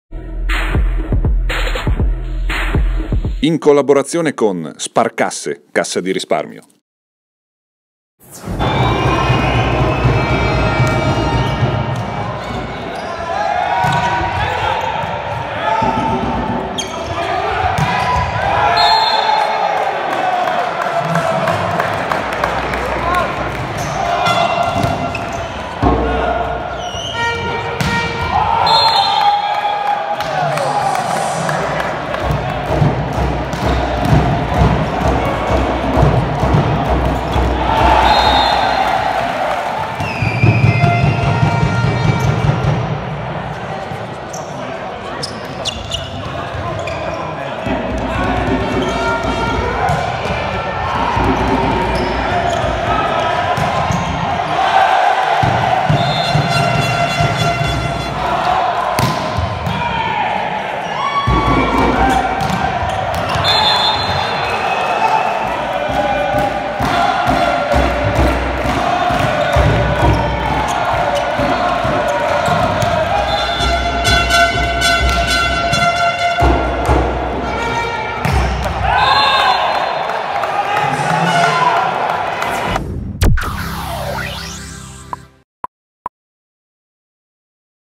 Highlights di Chaumont VB 52-Trentino Diatec 3-2 (Andata dei Playoffs 12 di Champions League)
Alcune immagini della gara d'andata dei Playoffs 12 di 2018 CEV Volleyball Champions League, giocata  al Complex Sportif Rene-Tys fra Chaumont VB 52 Haute Marne e Trentino Diatec il 14 marzo 2018.